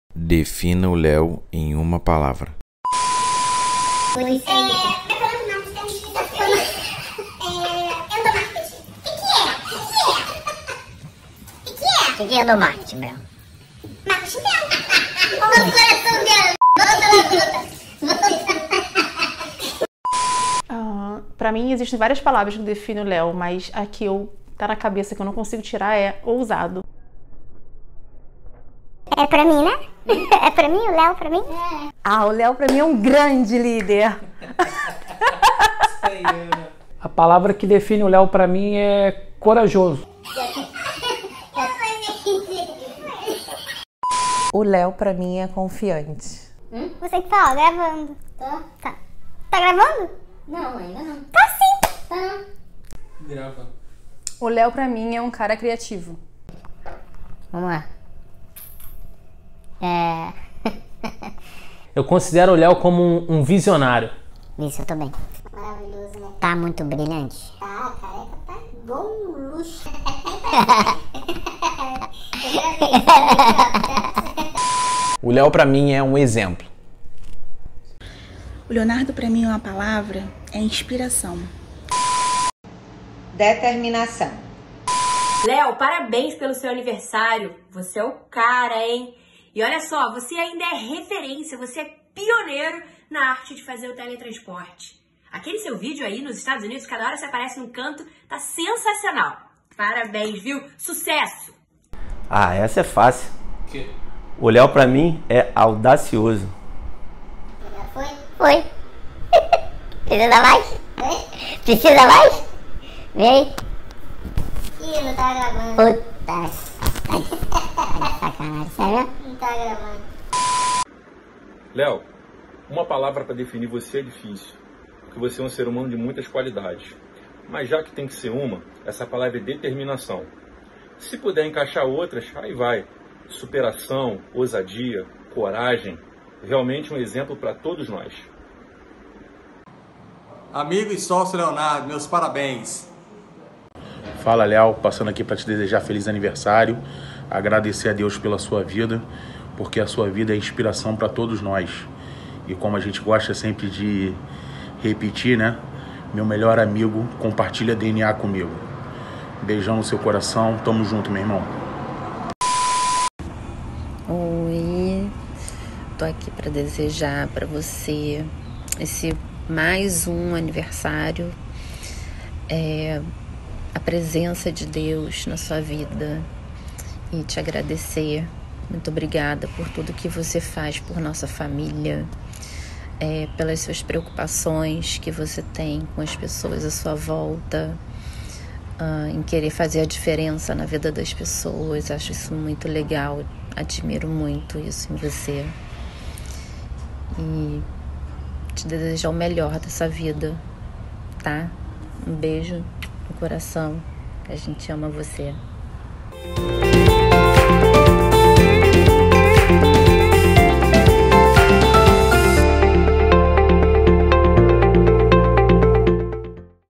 Feliz Aniversário Leo
Nossa equipe fez uma homenagem ao nosso CEO Dr. Leonardo Leão. Desejamos muito sucesso, sabedoria e saúde.